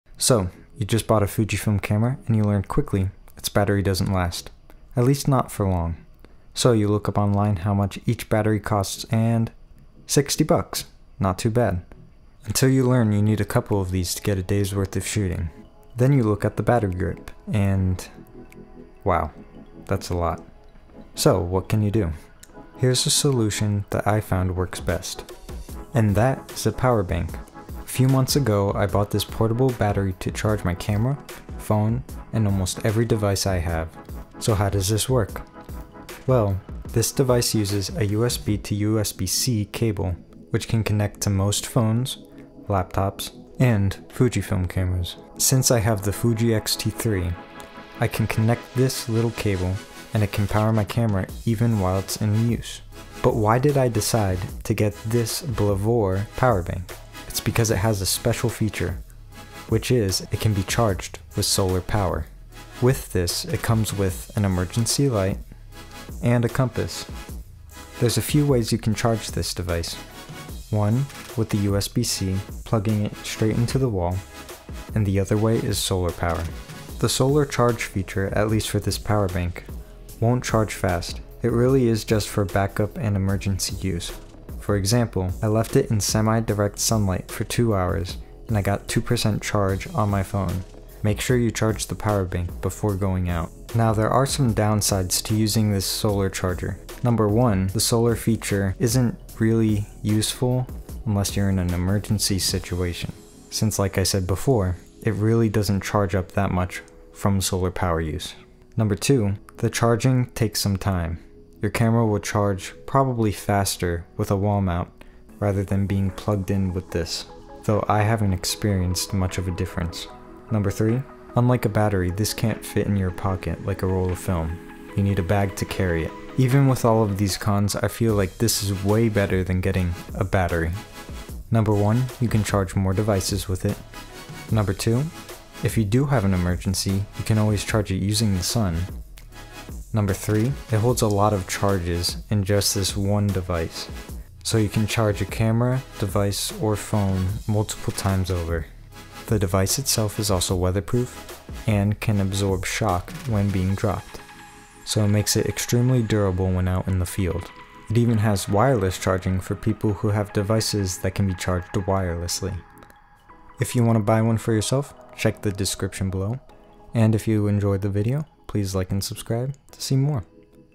Is this Better than Camera Batteries?
Do you want a cheaper alternative to camera batteries? Today I will cover how to get the best battery life for your Fujifilm camera on a budget.

In this video I will show you an alternative  to multiple batteries or an expensive battery grip. This device has 20,000 mAh, a solar panel, an emergency light, a compass, and wireless charging, and it's weather proof for field use. This battery can hold 11.1111... times more than the amount of power a Fujifilm battery can hold.

Blavor Solar Power Bank-

Blavor Solar Power Bank 2.0 -

CAMERA GEAR